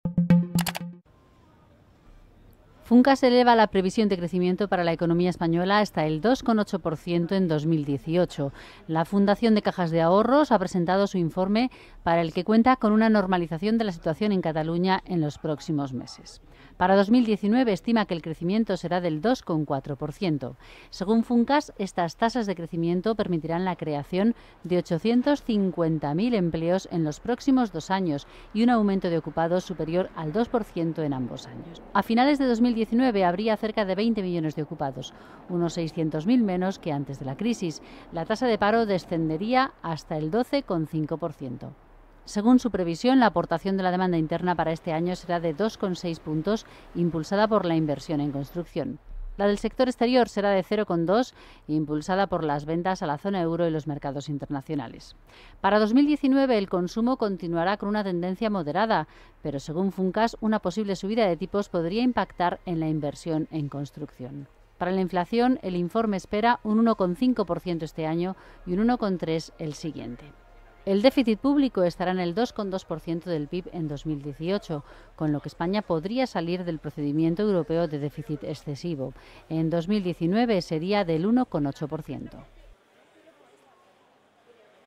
Funcas eleva dos décimas su previsión de PIB para 2018, hasta el 2,8 %
Madrid, 12 mar (EFE).- La Fundación de Cajas de Ahorros (Funcas) ha elevado hoy dos décimas su previsión de crecimiento de la economía española para 2018, hasta el 2,8 %, un pronóstico realizado bajo la hipótesis de una normalización de la situación en Cataluña en los próximos meses.
Según ha comunicado la Fundación, esta previsión para 2018 representa una desaceleración de tres décimas respecto al crecimiento registrado el año pasado (3,1 %) y ha estimado que se volverá a moderar hasta el 2,4 % en 2019.
Estas tasas de crecimiento permitirán la creación de 850.000 empleos en los próximos dos años, con aumentos del número de ocupados del 2,4 % en 2018, una décima más que en la anterior previsión, y un 2,1 % en 2019.
Así, a finales del próximo año, habría cerca de 20 millones de ocupados, unos 600.000 menos que antes de la crisis, y la tasa de paro descendería hasta el 12,5 %.
Dentro de la previsión de crecimiento del PIB para este año, Funcas señala que la aportación de la demanda interna será de 2,6 puntos y de 0,2 puntos la del sector exterior.
En el primer caso, el consumo privado acusará el agotamiento de la demanda embalsada durante la crisis, mientras que la inversión aumentará fruto de la recuperación de la construcción.
El sector externo aportará al crecimiento por el impacto favorable de la recuperación de las exportaciones en la zona euro y del crecimiento de los mercados internacionales.
De cara a 2019, el crecimiento del consumo continuaría la tendencia moderada, pero una posible subida de los tipos de interés podría impactar en la inversión, sobre todo residencial, algo que también afectaría a otros países europeos, lo que reducirá ligeramente la aportación del sector exterior.
En cuanto a la inflación, Funcas espera un incremento medio del IPC del 1,5 % este año, una décima menos que en la anterior previsión, y un 1,3 % en 2019, pese al encarecimiento del petróleo en los últimos meses.
El déficit público se situará en el 2,2 % del PIB en 2018, en línea con el objetivo, lo que permitirá a España salir del procedimiento europeo de déficit excesivo, mientras que en 2019 bajaría hasta el 1,8 % del PIB y la deuda pública se situaría en el 94,8 % del PIB.
Entre los principales desafíos para la economía española, Funcas destaca el ritmo al que los bancos centrales retiren los estímulos monetarios y, en el plano interno, además de la incertidumbre en Cataluña, considera un riesgo la parálisis de las reformas y del esfuerzo de contención de la deuda pública.